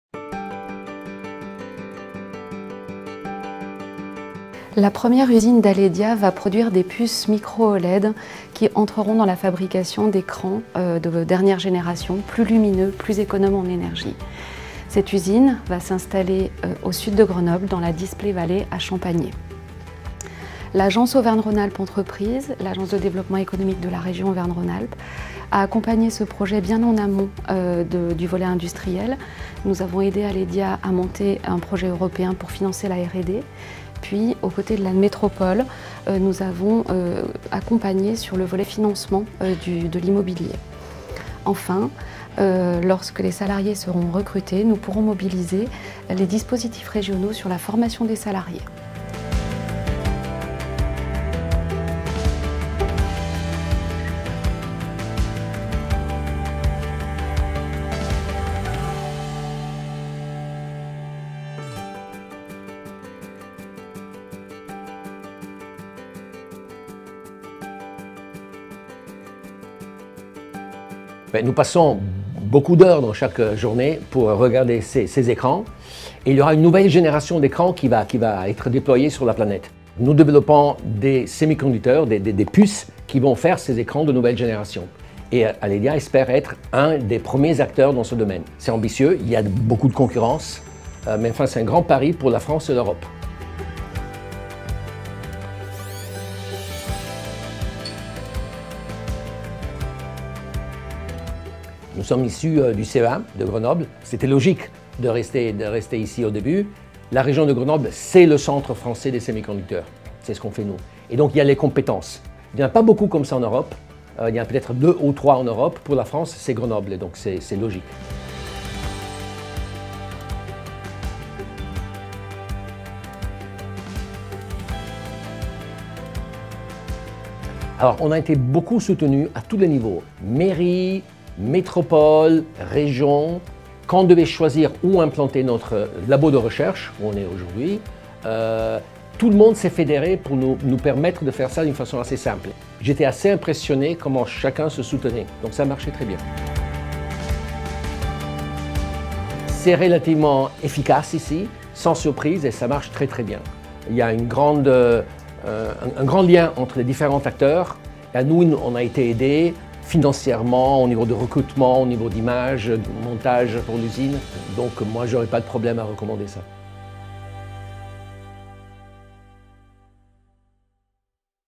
[Interview] 🎤 Giorgio Anania d'ALEDIA EN
L'usine d'Aledia va produire des puces microLED qui vont entrer dans la fabrication d'écrans de dernière génération, plus lumineux et plus économes en énergie.

L'agence Auvergne-Rhône-Alpes Entreprises  a accompagné ce projet bien en amont du volet industriel. Nous avons aidé Aledia a monté un projet européen pour financer la R&D. Puis, aux côtés de la Métropole, nous avons accompagné sur le volet financement de l'immobilier. Enfin, lorsque les salariés seront recrutés, nous pourrons mobiliser les dispositifs régionaux sur la formation des salariés.

Vidéo : KabochArts